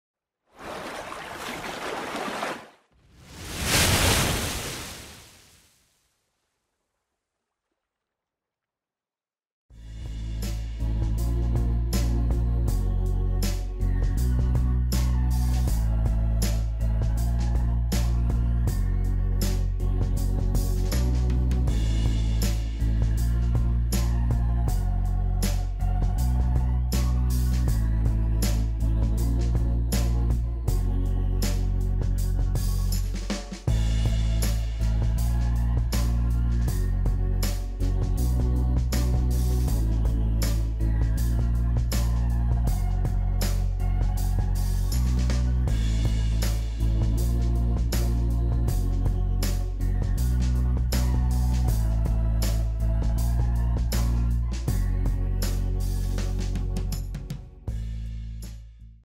1/17 Mistletoe FOR SALE!
2 Bedroom Duplex in a Quiet location in Kingscliff for Sale. Great value property in a competitive market.